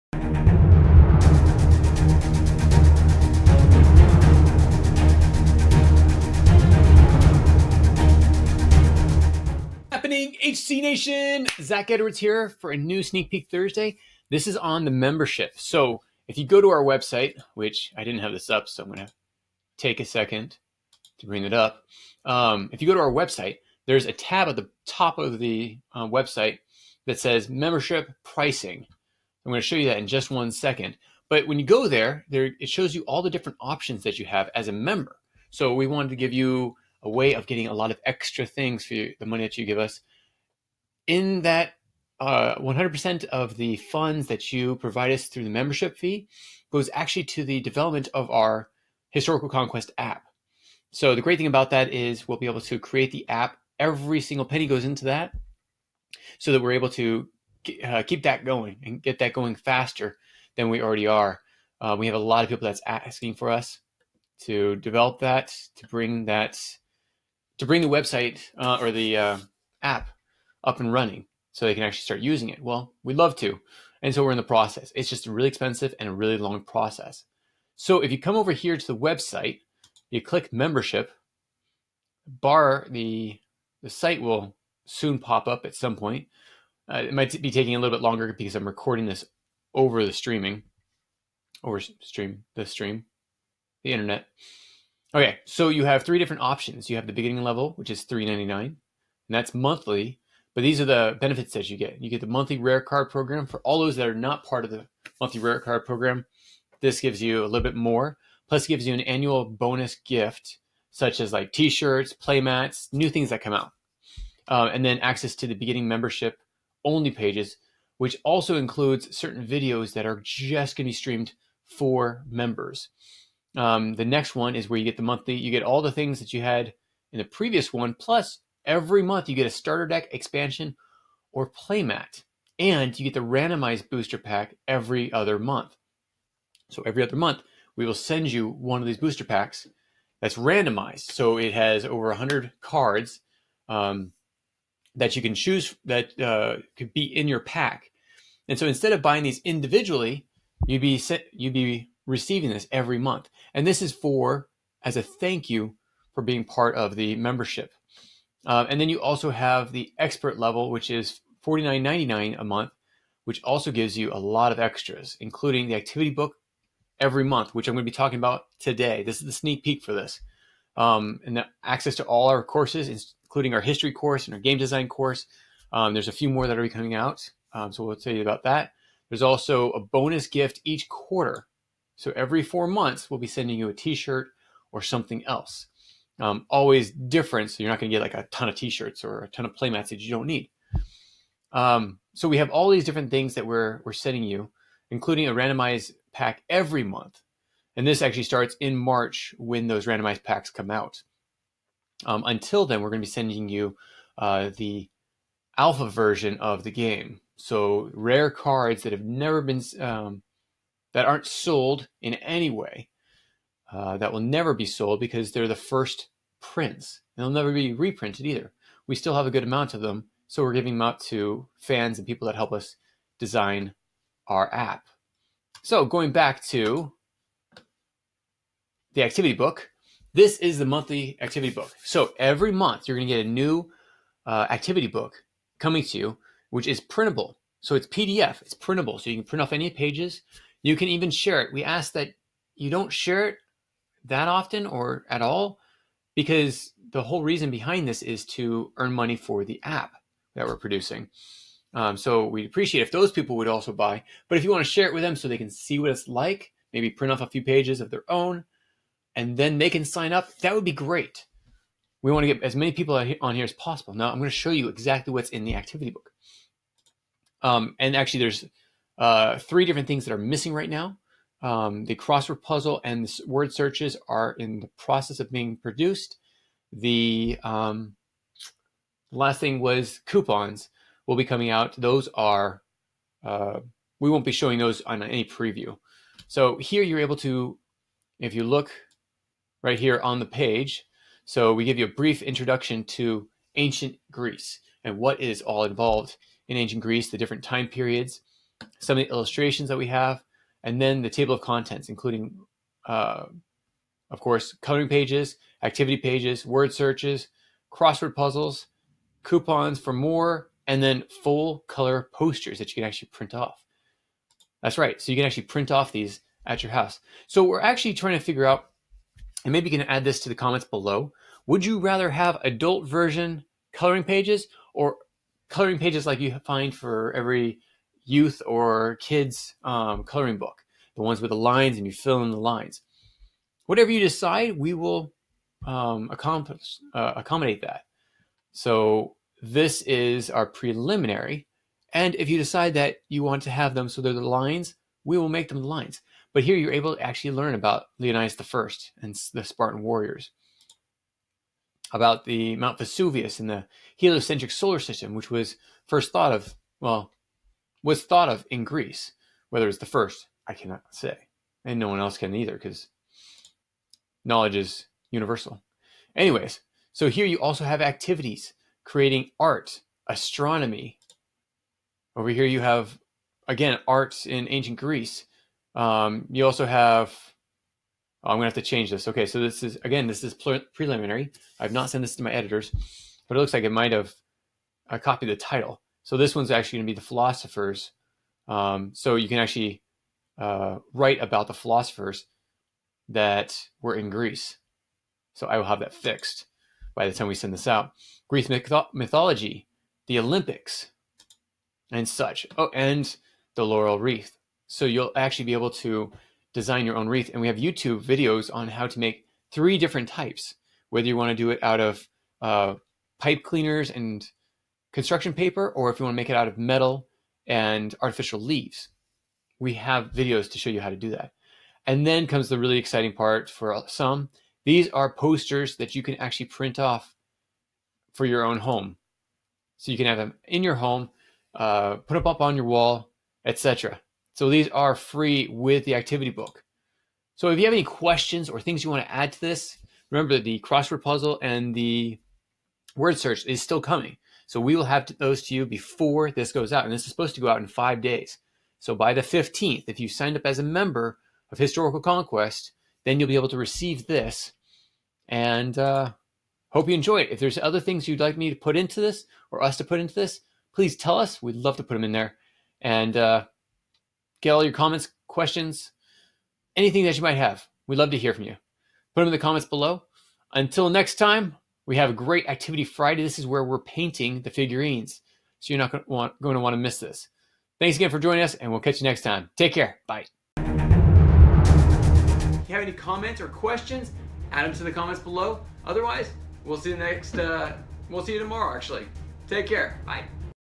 Sneak Peak Thursday - Monthly Membership Activity Book Greeks (1/10/2019)
The monthly activity book is almost complete. This is a sneak peak of the content minus the crosswords, word search, and coupons found within. Take a peak.